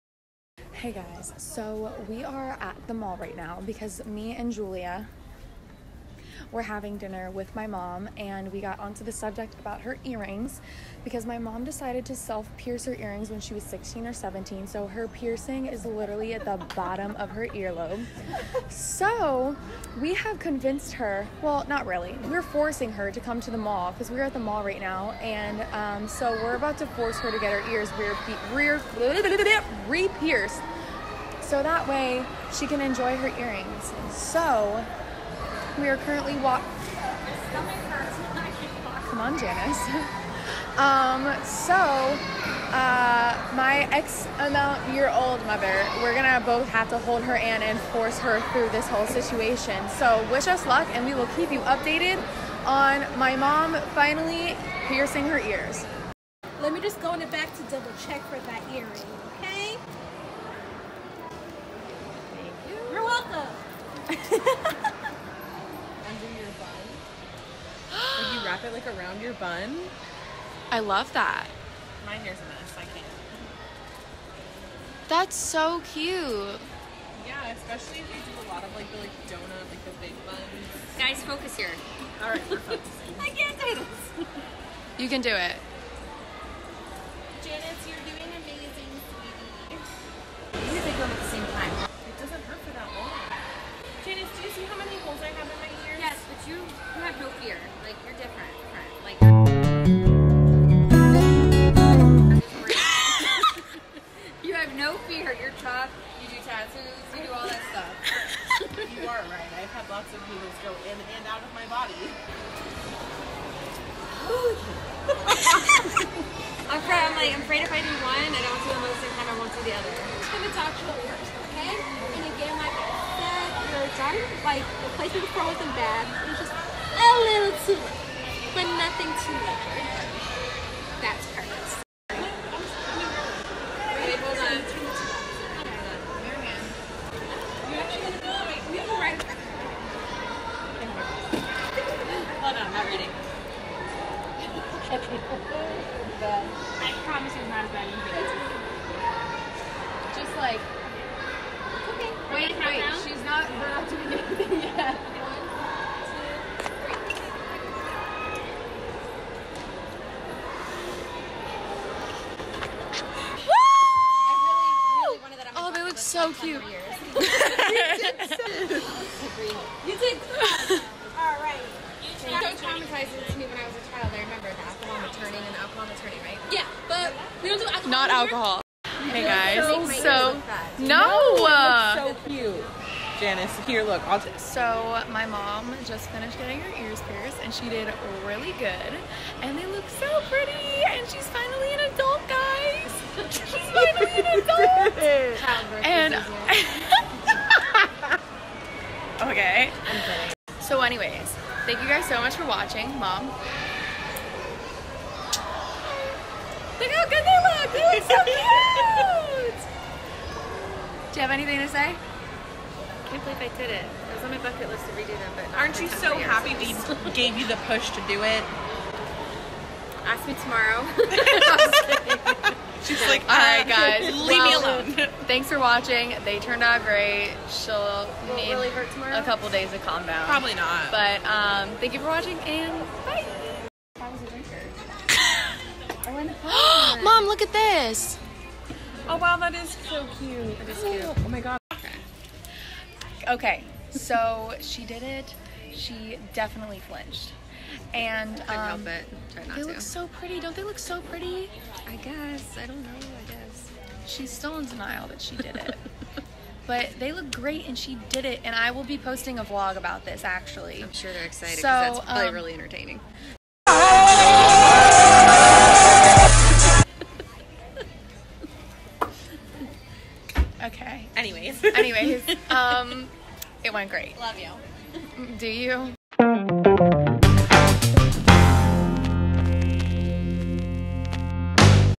mom finally gets her ear pierced - vlog
gonna try to start doing more of these! Let me know if ya like!!
Comment if you think they look good!!